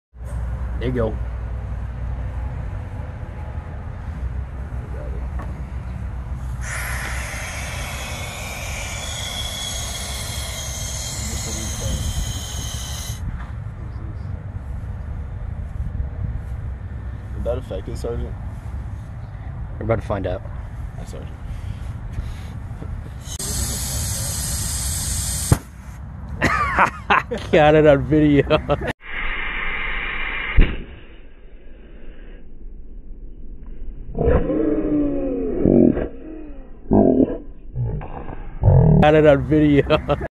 Balloon Explosion 🎈🎉
Attempting to inflate a dry rotted balloon.